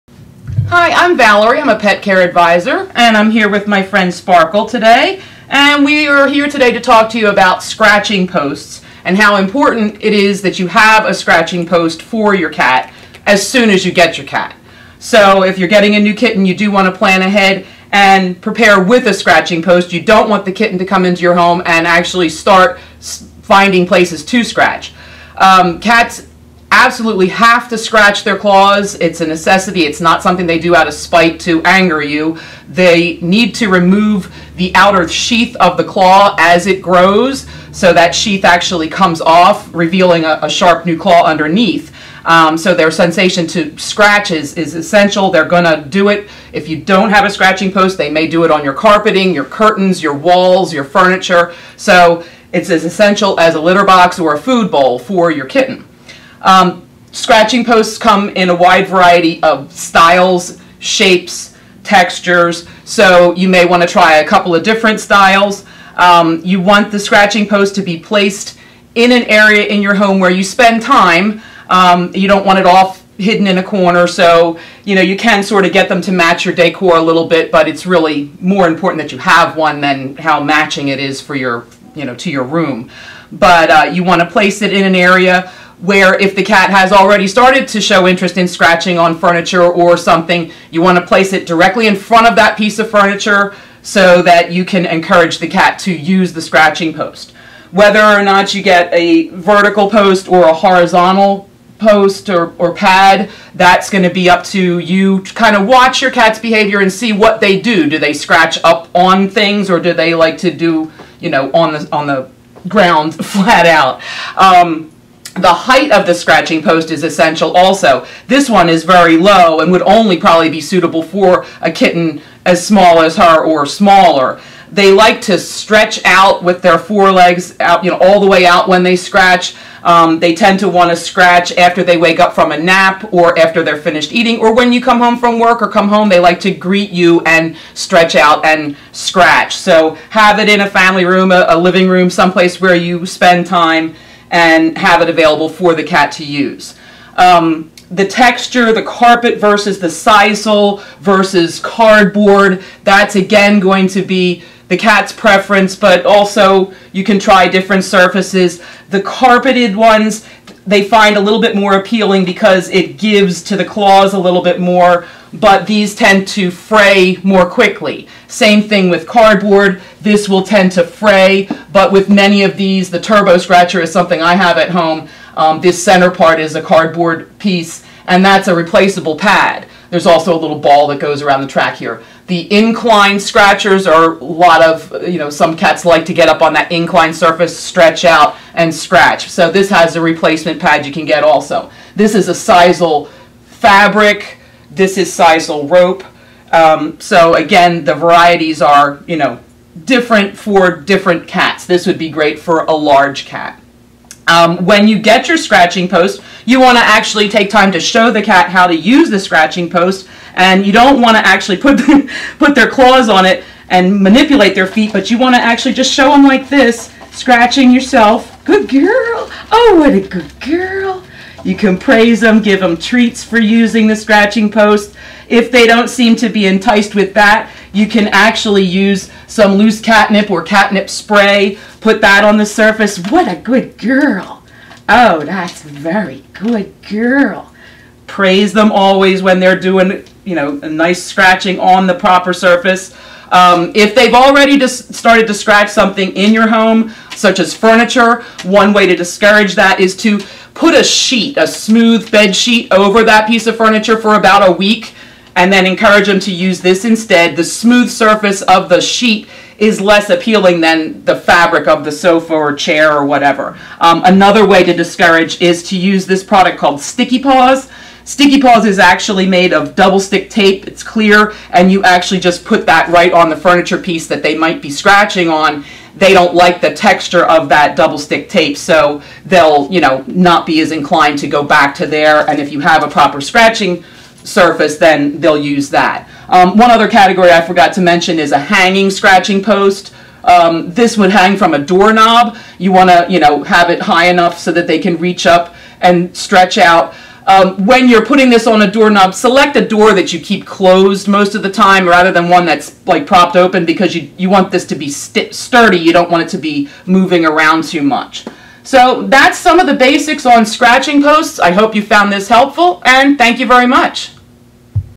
Selecting a Scratching Post for Your Cat
Cats need to have a proper surface to scratch on. If you fail to provide your cat with a scratching post of pad, they will inevitably  find something to scratch on such as your carpet or furniture. Pet-Care Advisor, Valerie shows some of the different styles and textures of scratching posts you can choose from. To read more about this subject, you can visit the original blog posting here: